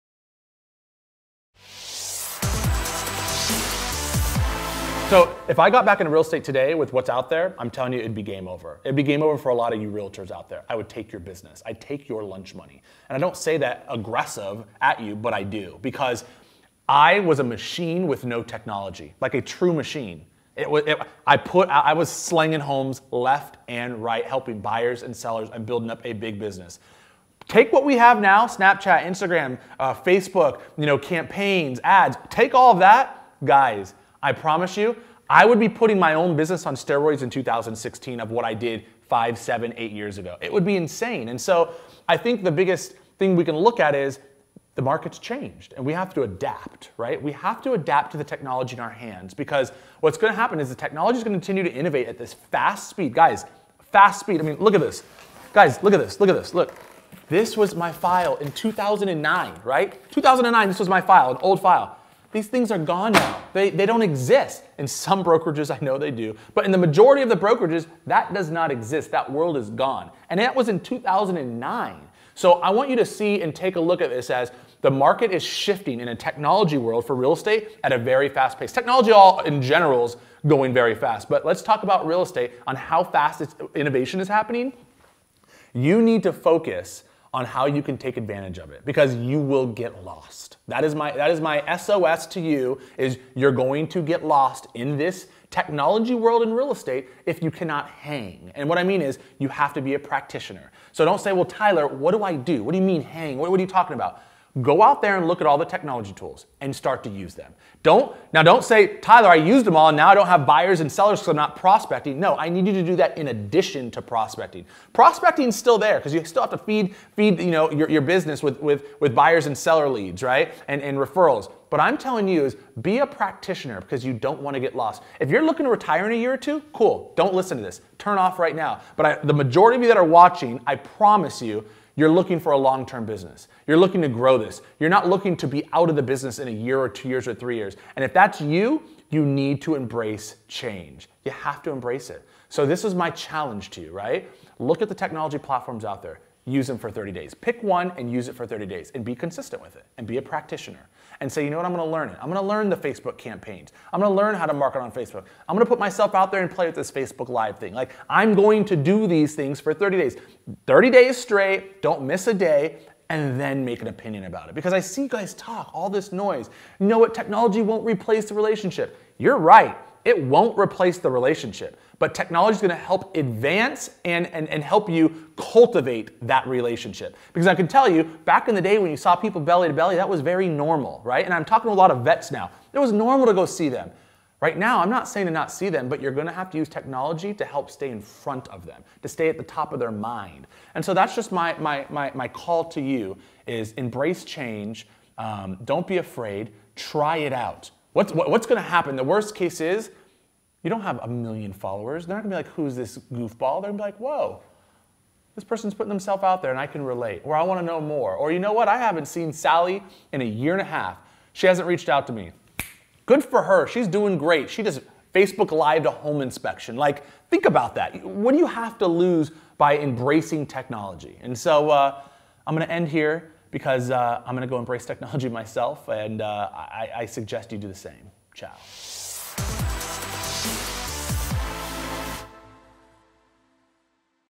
How To Take Advantage of Technology and Embrace Change
It's clear that in just a handful of years, the market has changed. What does that mean for Realtors? It means we have to embrace change.